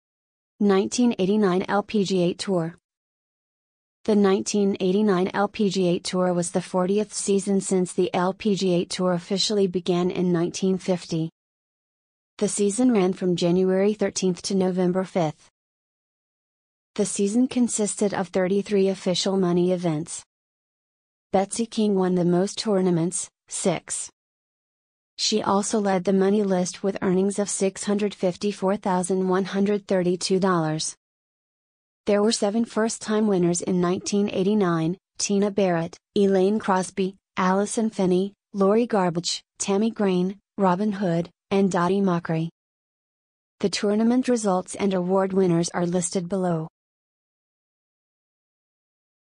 1989 LPGA Tour
1989 LPGA Tour

The 1989 LPGA Tour was the 40th season since the LPGA Tour officially began in 1950. The season ran from January 13 to November 5. The season consisted of 33 official money events. Betsy King won the most tournaments, six. She also led the money list with earnings of $654,132.
There were seven first-time winners in 1989: Tina Barrett, Elaine Crosby, Allison Finney, Lori Garbacz, Tammie Green, Robin Hood, and Dottie Mochrie.
The tournament results and award winners are listed below.

#1989 LPGA Tour wikipedia facts fact This video generated Wikipedia by Bot, Sorry for the irrelevant picture or video